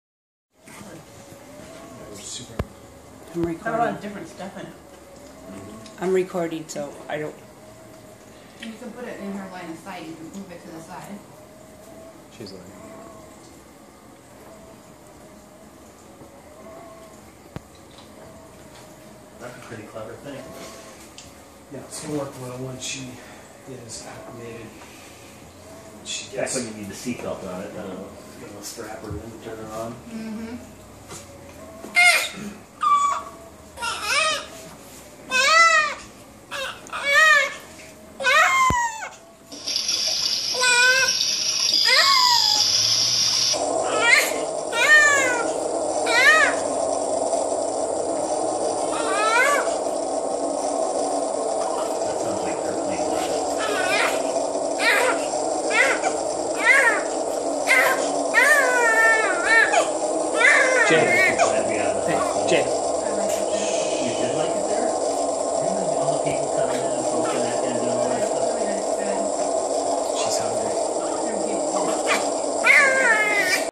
Jaeda in her moving chair
Jaeda is only a couple days old